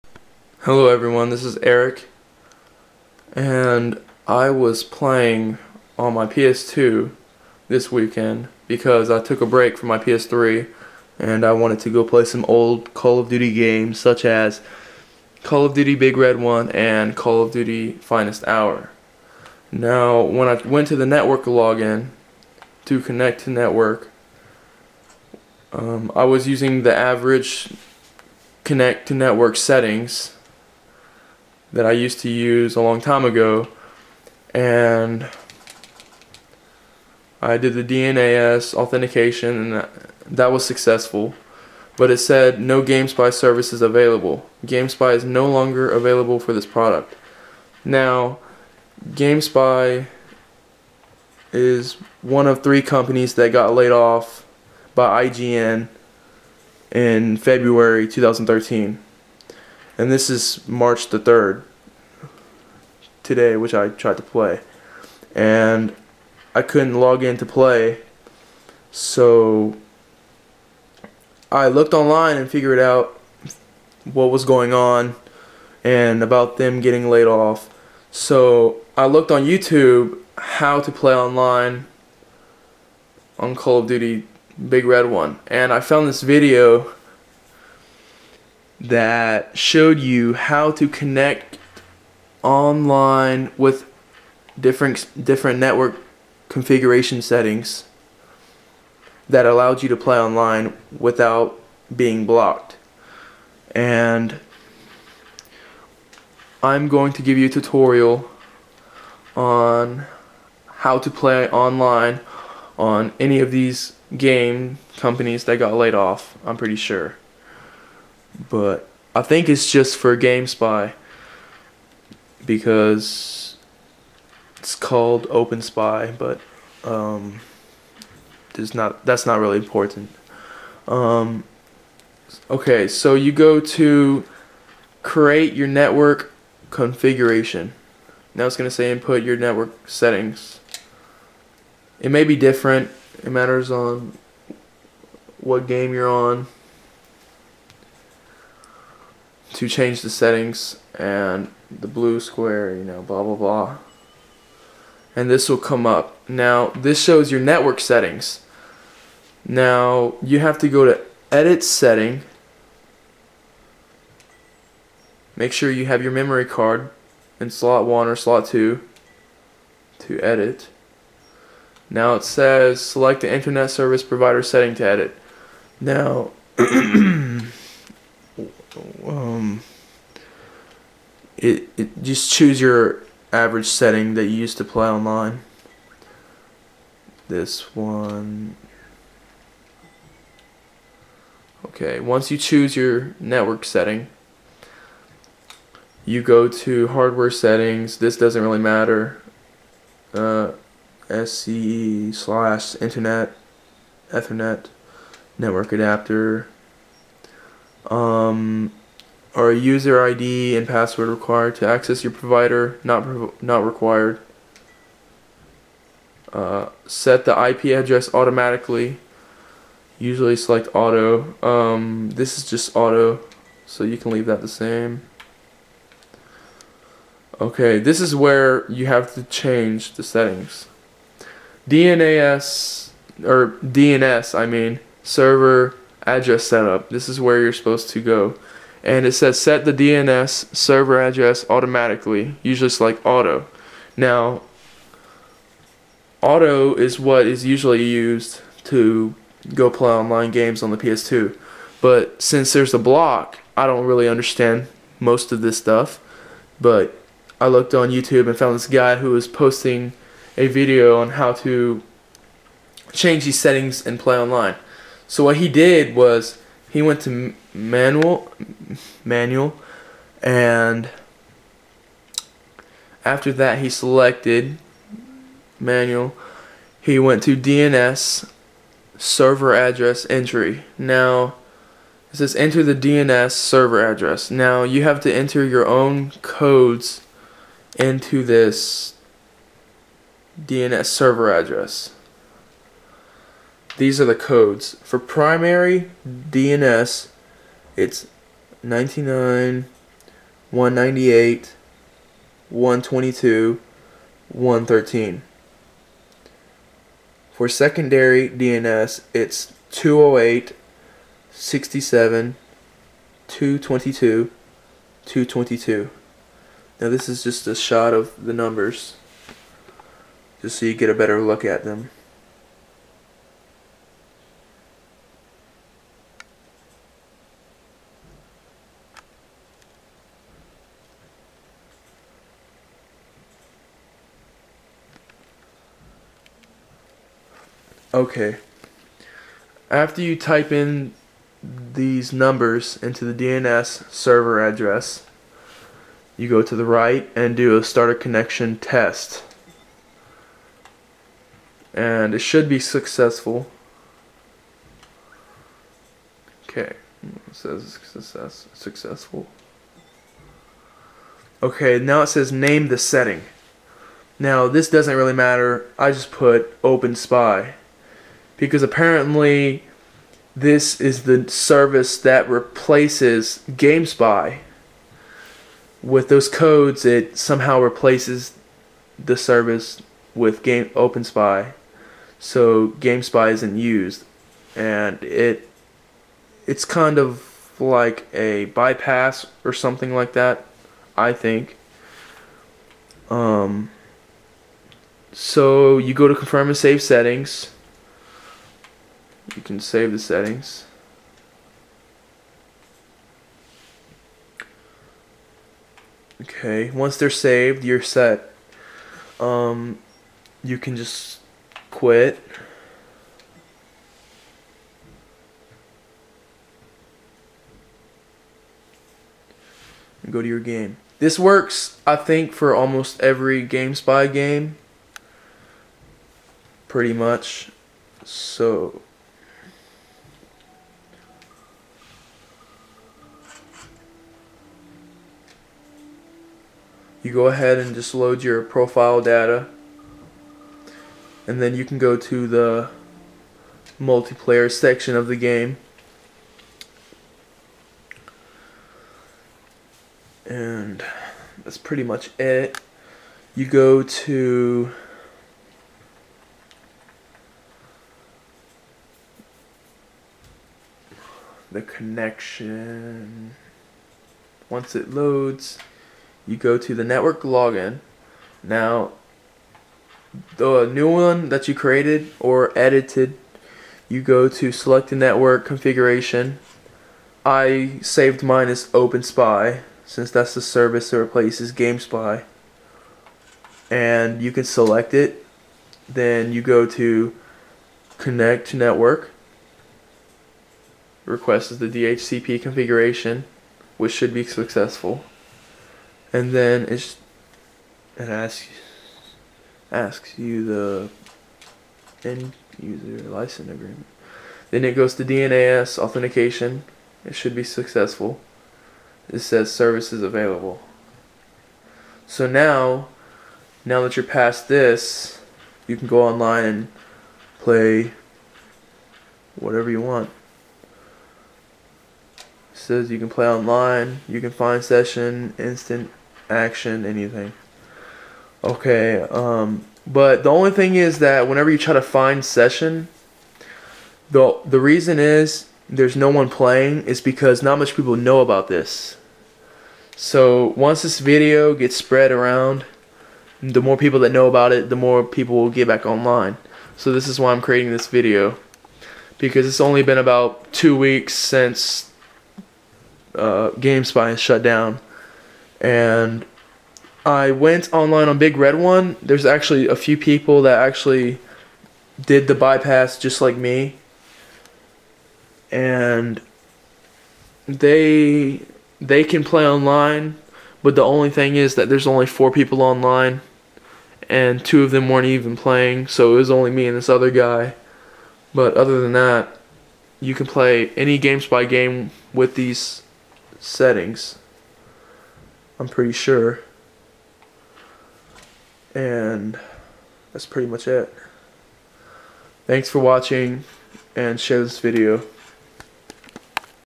How to play online with ps2 Gamespy games after shut down of service.
This video teaches you how to play online with ps2 Gamespy games after the shut down of the video game service. The difficulty to do this is easy. Go to network settings and type in the numbers shown in the dns settings. Then your done. Save settings and enjoy online gameplay of the new Openspy network.